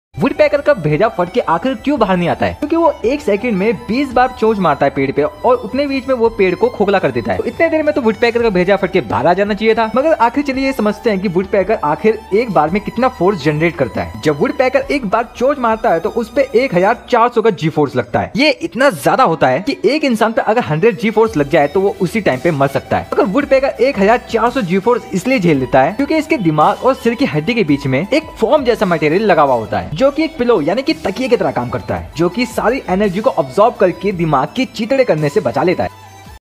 How Woodpeckers Survive 1400g force
This is how woodpeckers resists head impact injury.